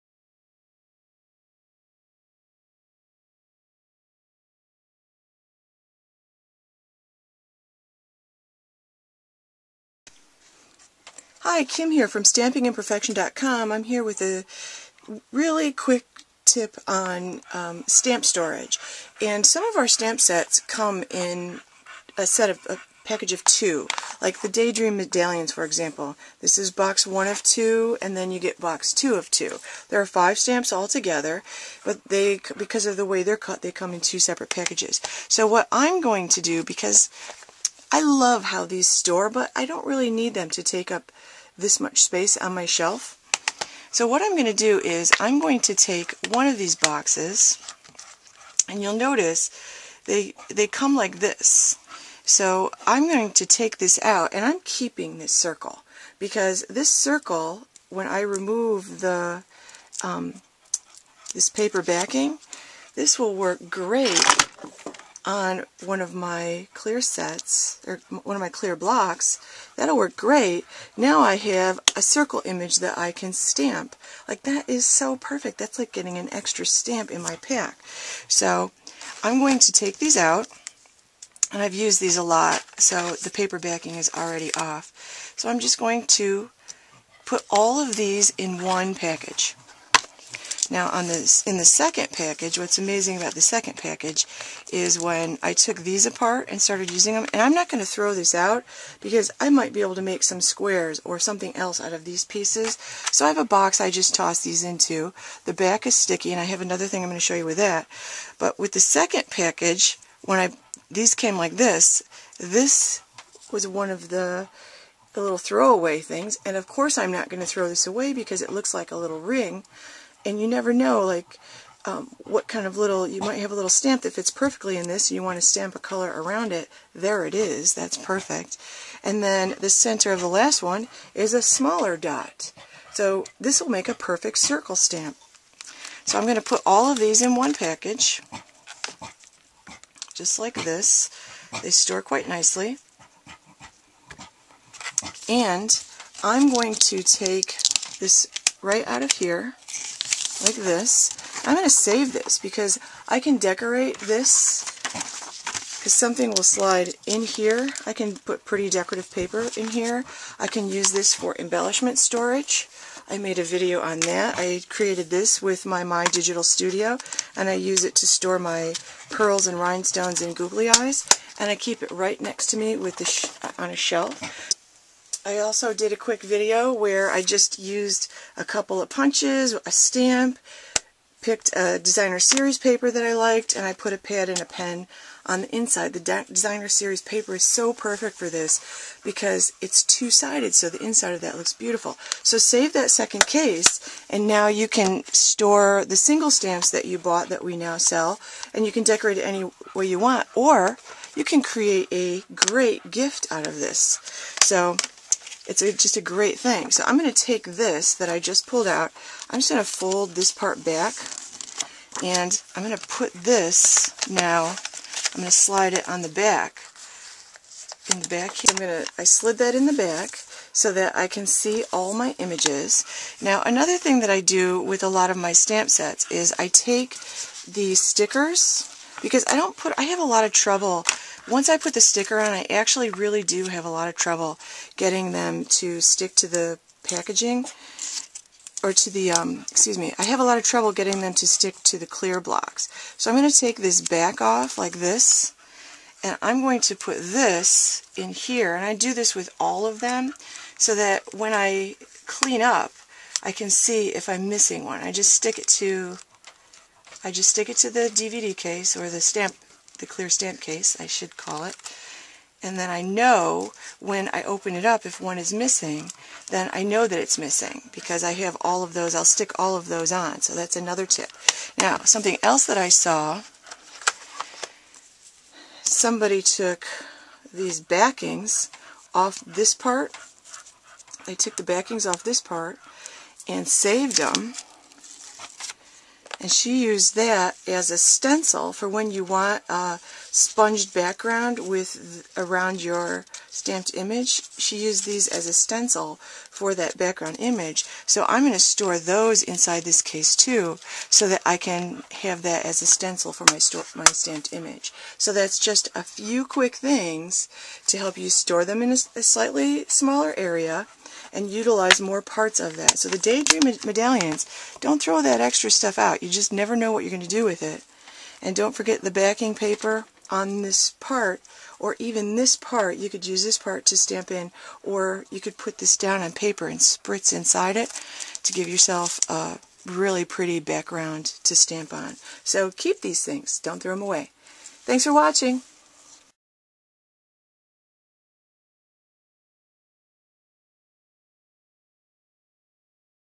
Stampin' Up! Two Minute Quick Tip: Stamp Storage
Make better use of your clear mount stamp storage!  to sign up for the mailing list and get email newsletters full of project instructions, ideas, and tips!